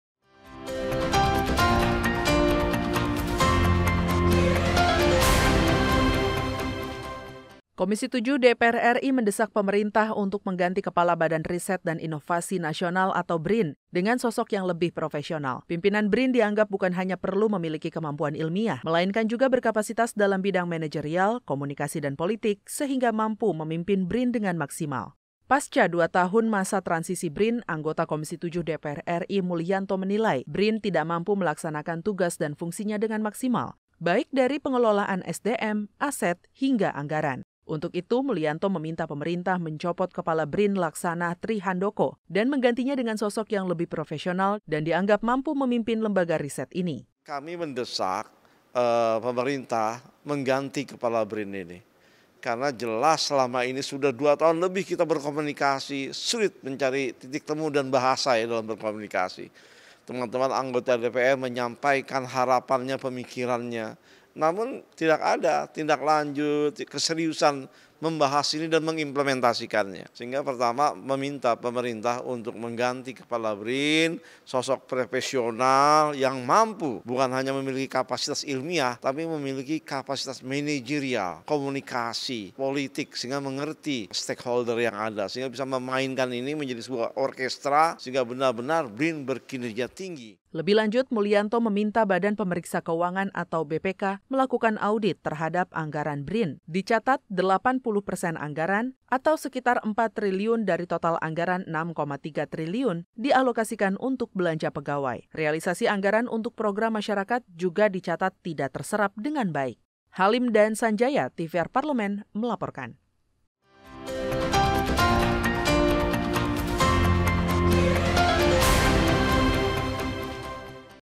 TVR 120 - Komisi VII DPR RI: Desak Pemerintah Ganti Kepala BRIN
Komisi VII DPR RI mendesak pemerintah untuk mengganti Kepala Badan Riset dan Inovasi Nasional atau BRIN dengan sosok yang lebih profesional. Pimpinan BRIN dianggap bukan hanya perlu memiliki kemampuan ilmiah, melainkan juga berkapasitas dalam bidang manajerial, komunikasi dan politik, sehingga mampu memimpin BRIN dengan maksimal.

Narasumber:
Mulyanto / Anggota Komisi VII DPR RI / F-PKS/ Banten III